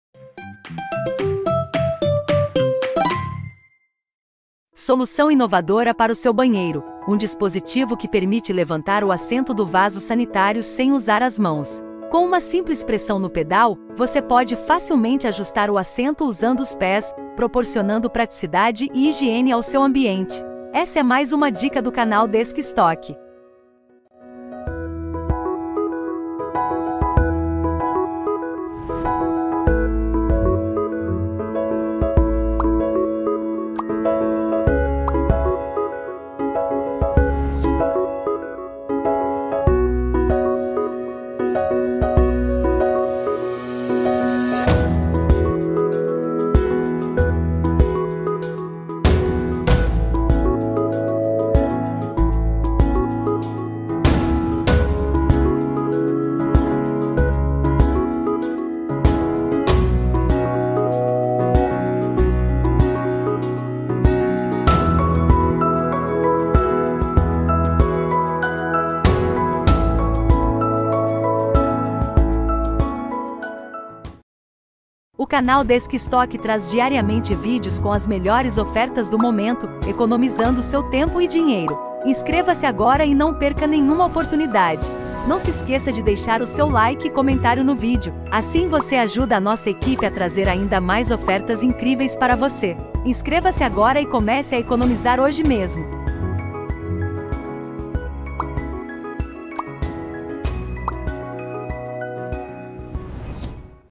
DISPOSITIVO PEDAL PARA LEVANTAR ASSENTO VASO SANITÁRIO SEM AS MÃOS
Solução inovadora para o seu banheiro: um dispositivo que permite levantar o assento do vaso sanitário sem usar as mãos. Com uma simples pressão no pedal, você pode facilmente ajustar o assento usando os pés, proporcionando praticidade e higiene ao seu ambiente.
Essa é mais uma dica do canal Desk Stok!